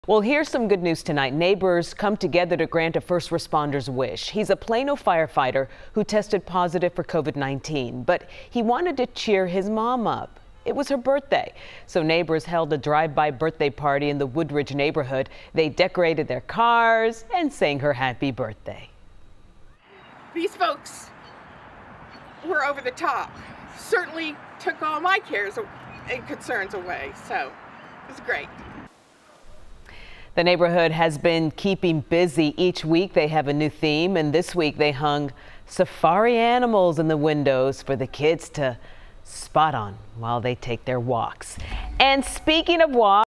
Neighbors come together for firefighter sickened by COVID-19
It was his mom's birthday, but he couldn't be with her. So his community made sure she still felt his love.